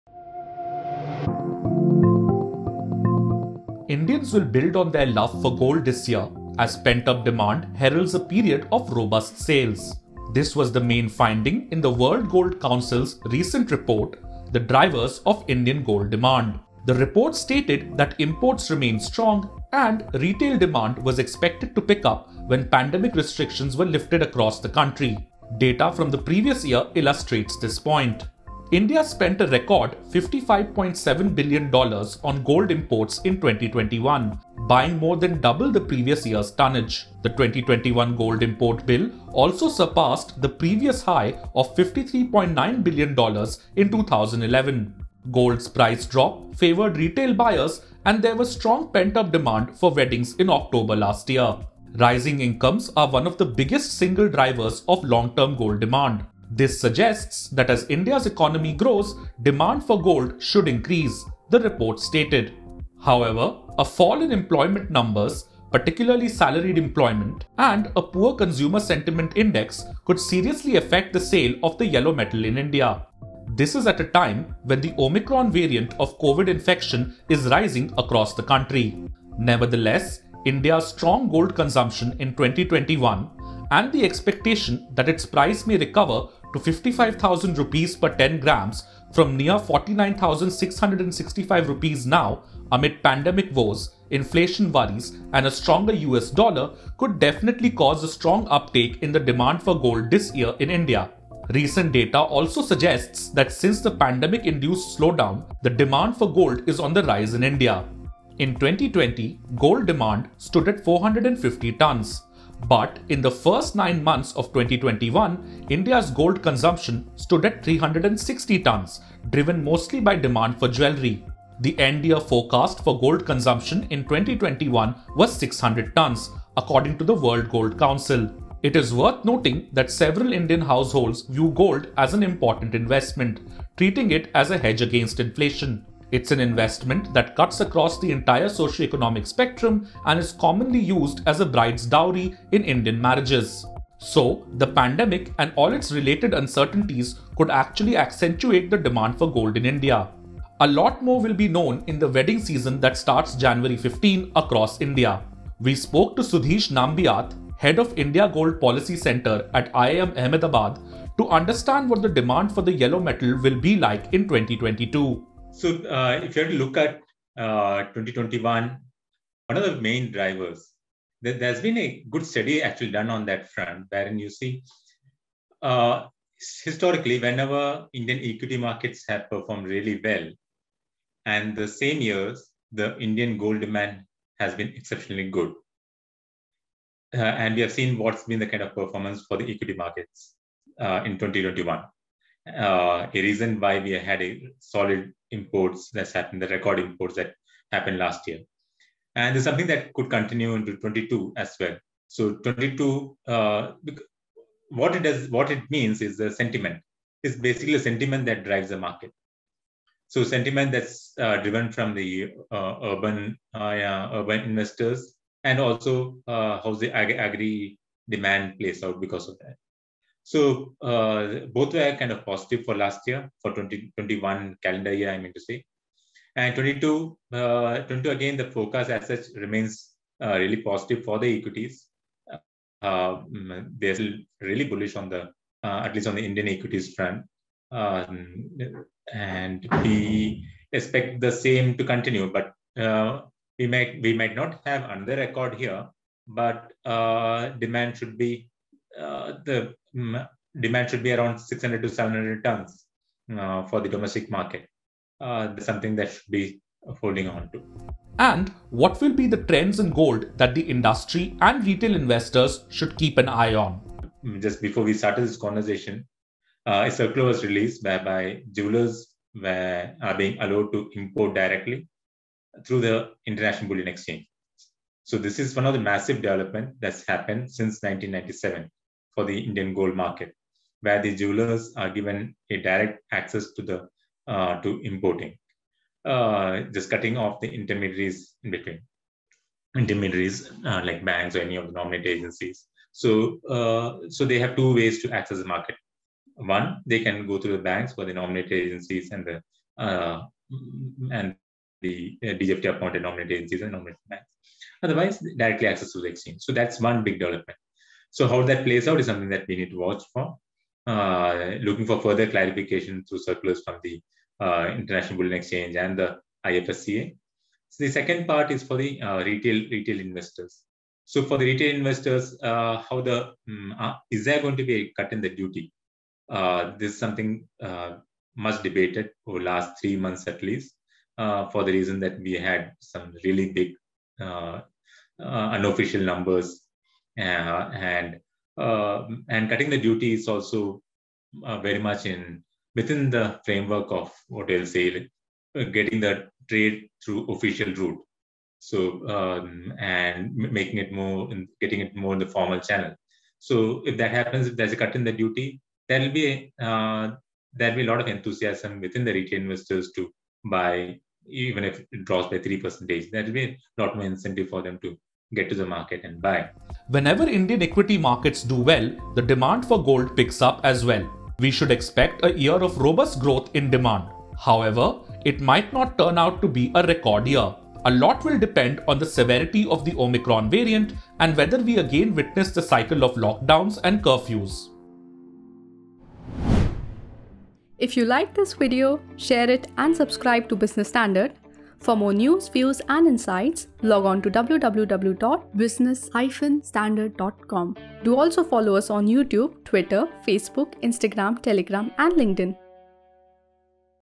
What is behind India’s renewed love for gold?
India imported 1,050 tn gold worth ($55.7 bn) in 2021, more than double of 2020. Last year’s gold import surpassed the high recorded in 2011. Find out what is behind India’s renewed rush for gold?